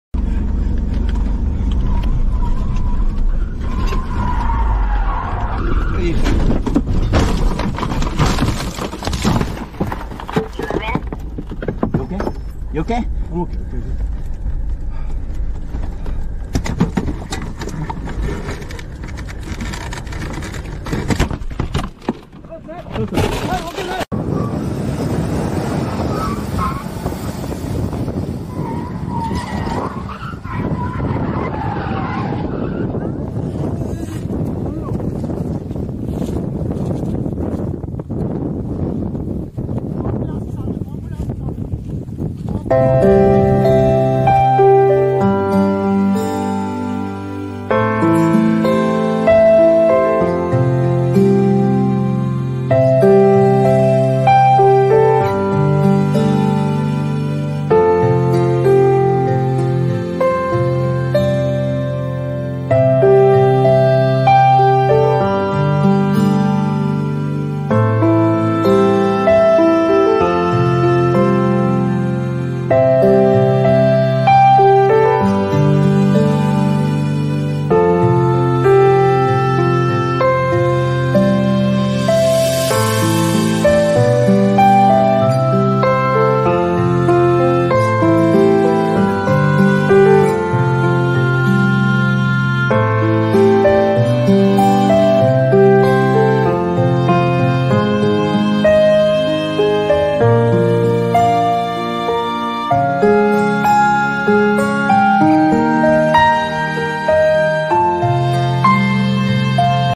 Watch the unexpected car crash involving Ajith Kumar during the filming of Vidaa Muyarchi
Witness the unforeseen moment as Ajith Kumar encounters a car crash on the set of Vidaa Muyarchi. This incident, captured during filming, offers a glimpse into the unpredictable nature of production environments. Despite rigorous safety measures, accidents can still occur, reminding us of the risks involved in the entertainment industry. Ajith Kumar's presence amidst the unexpected event adds a layer of intrigue to the footage, sparking discussions among fans and industry insiders alike.